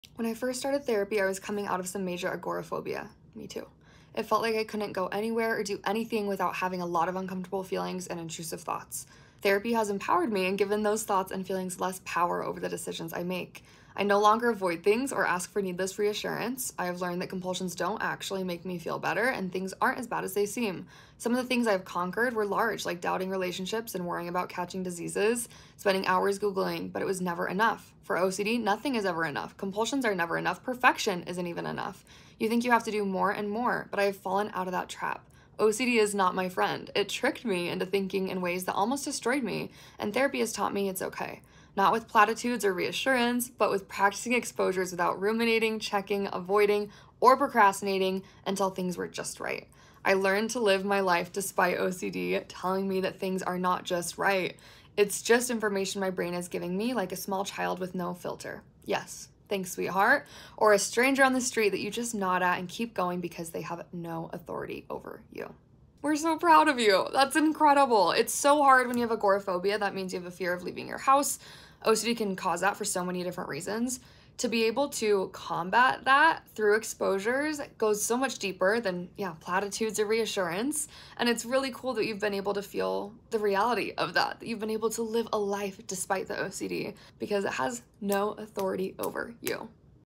Reading your Success Stories
We are so proud of all of our members using therapy to combat their OCD! When our members achieve a significant improvement in their OCD symptoms and quality of life, we recognize them as OCD Conquerors.

We created this milestone to celebrate how far you’ve come. Becoming an OCD Conqueror means that you're equipped to take on life's ups and downs, knowing that they can't undo your progress or take away the tools you've developed to manage OCD.

We're so grateful to all of our Conquerors who've shared their experiences and what therapy has made possible for them. We want to continue featuring your stories to keep spreading awareness and hope in our community.